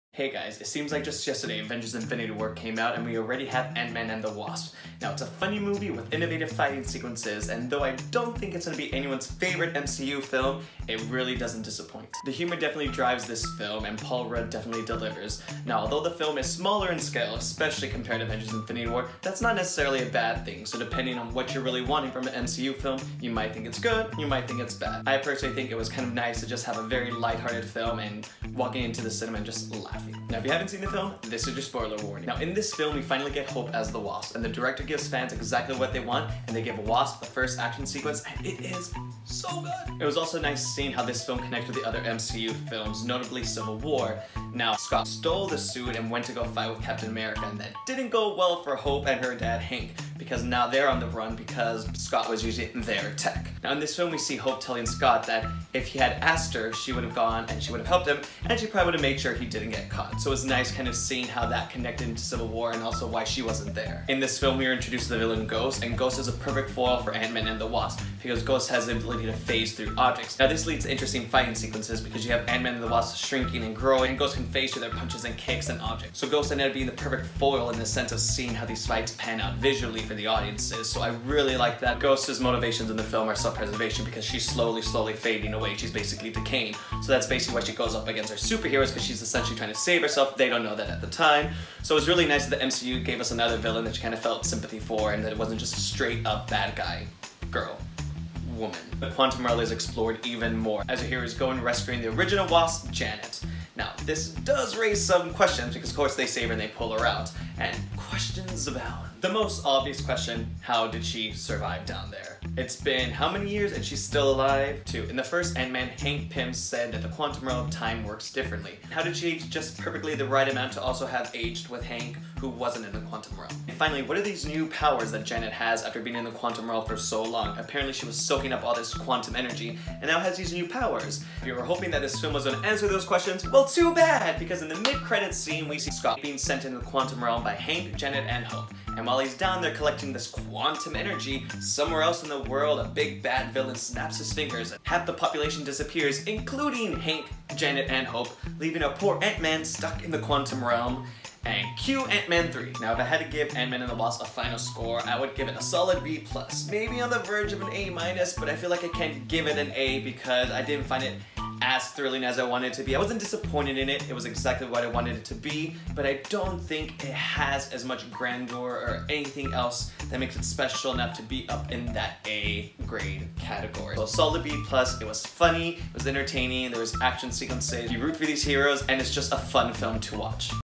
Ant-Man and the Wasp Review
Hey guys! Sorry for the portrait mode. I recorded this specifically for my IGTV channel on Instagram but didn't want to leave out my YouTube base!

Watch my thoughts on the latest MCU installment Ant-Man and the Wasp!